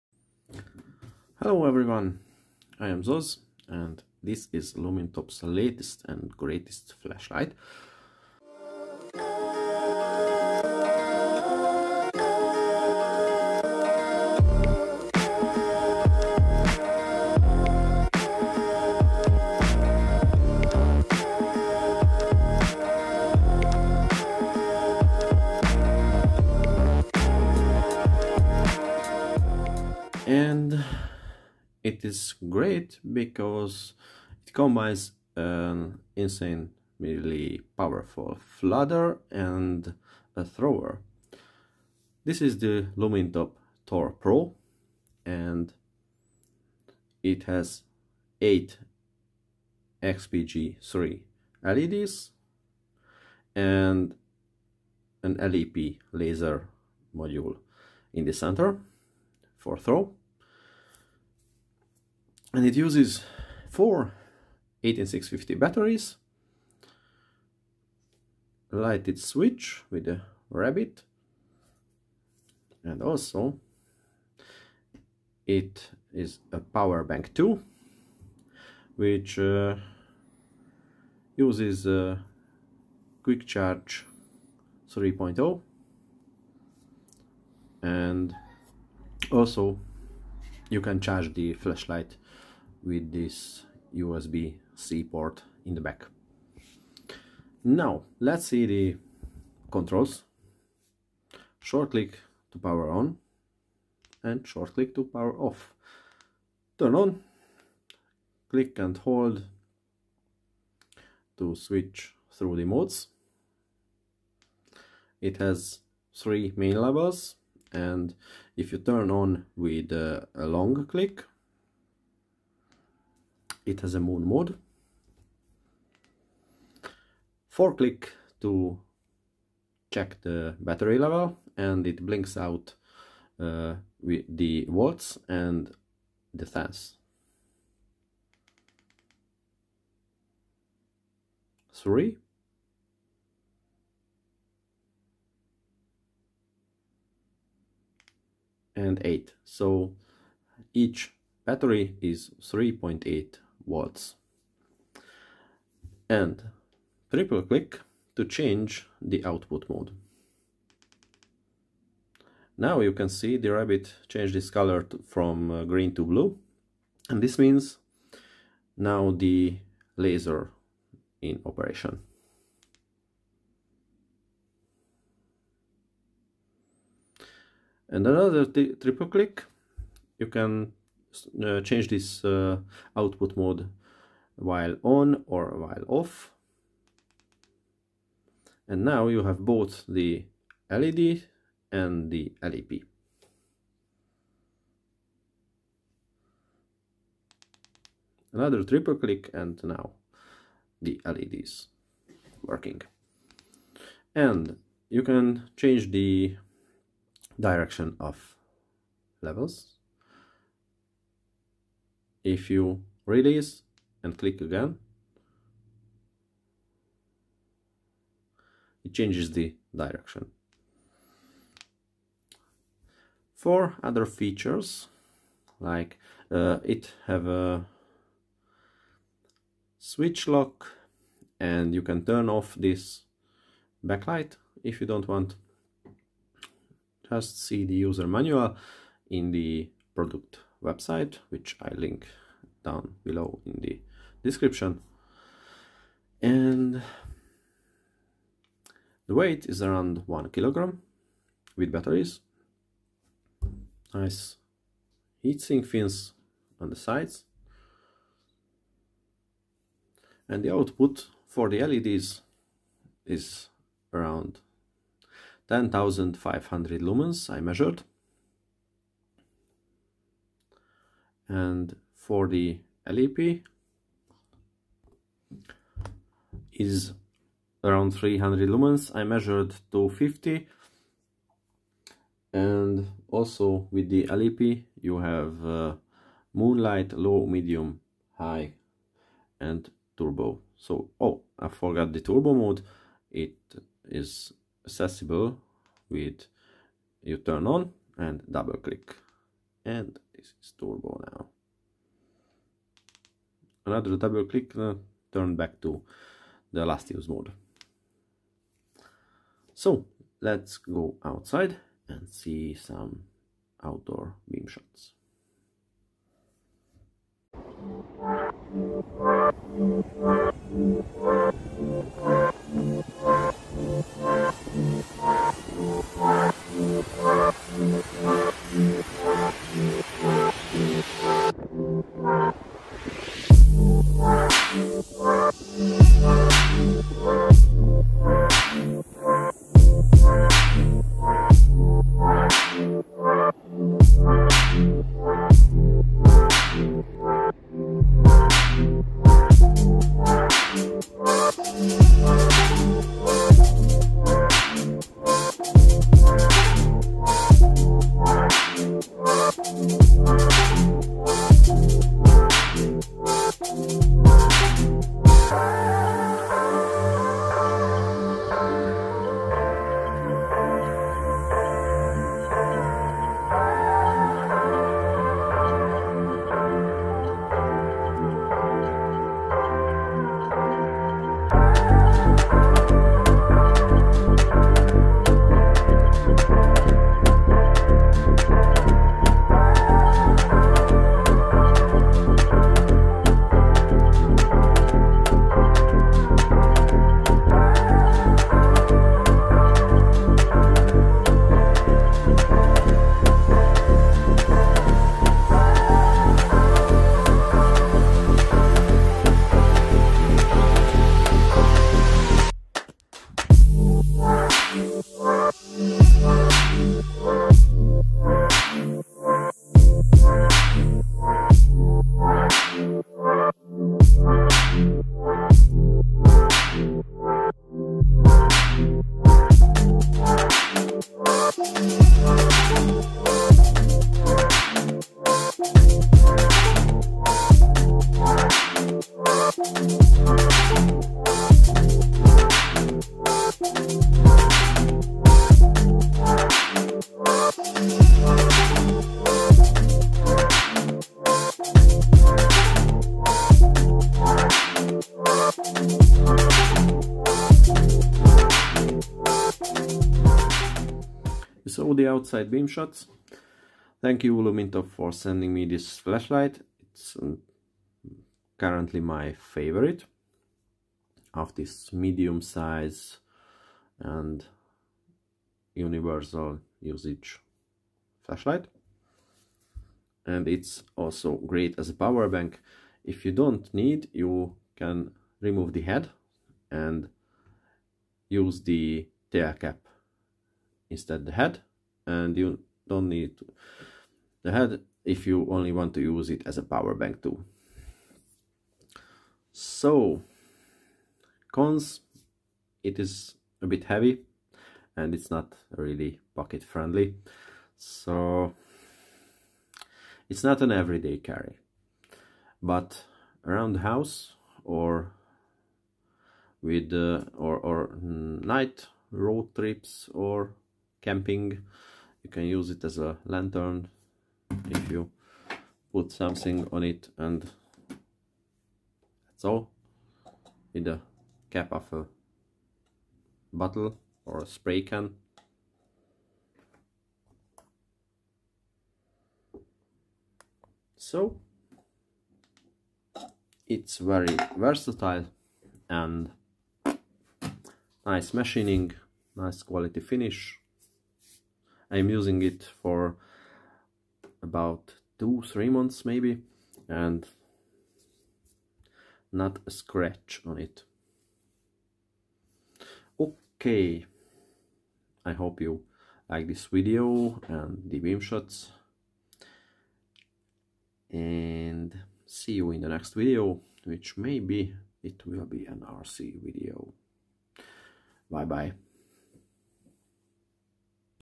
12000 lumens Laser(LEP)/Led flashlight from Lumintop
My test product was an early release prototype. the one you can buy uses XP-L HD Leds instead of XP-G3 Leds.

This is my honest opinion about the product. I didn't get any guidelines from Lumintop how I made the review or what can I say in it! I didn't get any payment to make this review other than I can keep the flashlight.